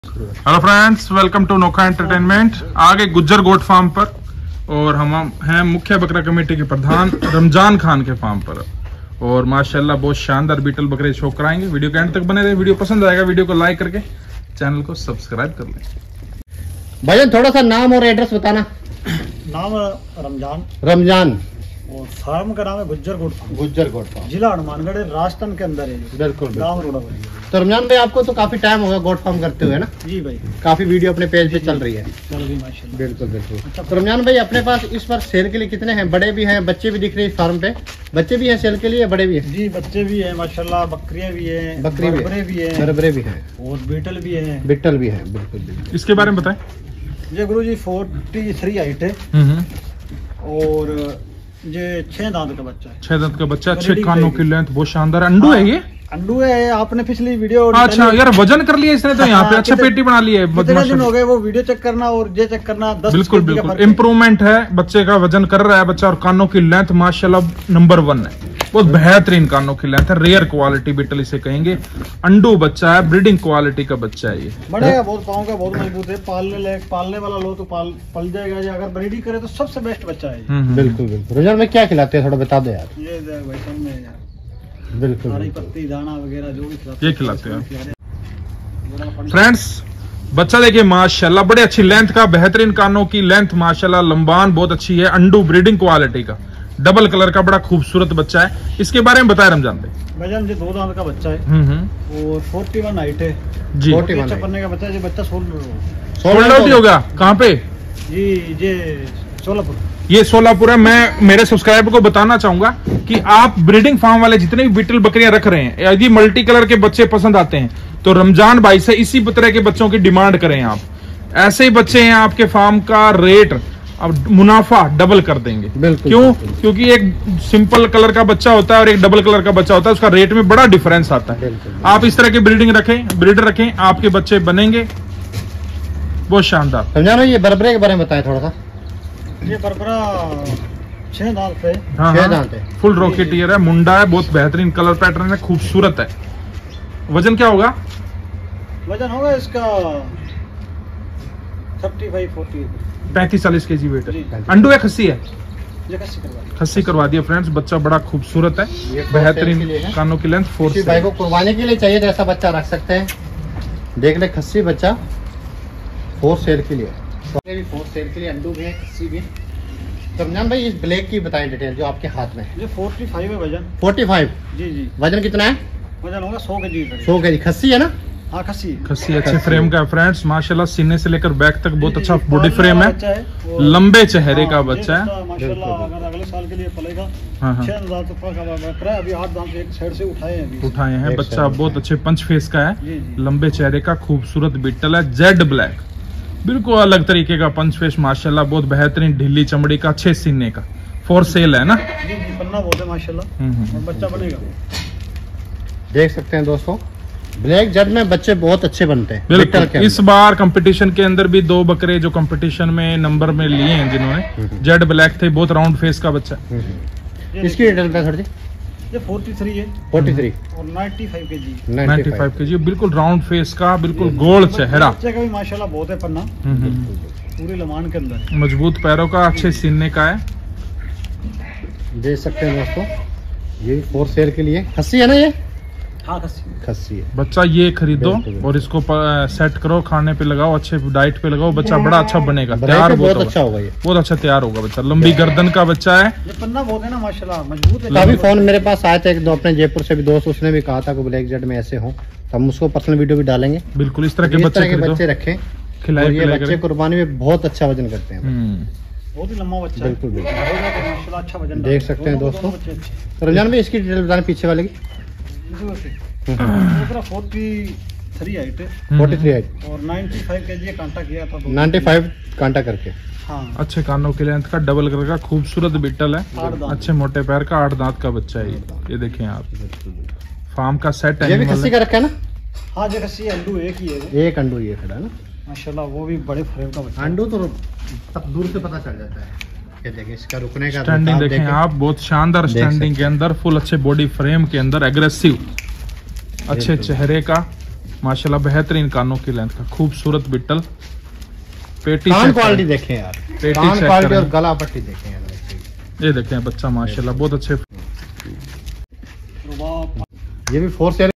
बीटल नस्ल के बकरे का कलेक्शन गुज्जर गॉट फार्म हनुमानगढ़ राजस्थान पर Beetal Male Goats Lot
बीटल नस्ल के बकरे का कलेक्शन गुज्जर गॉट फार्म हनुमानगढ़ राजस्थान पर Beetal Male Goats Lot at Gujjar Goat Farm Hanumangarh Rajasthan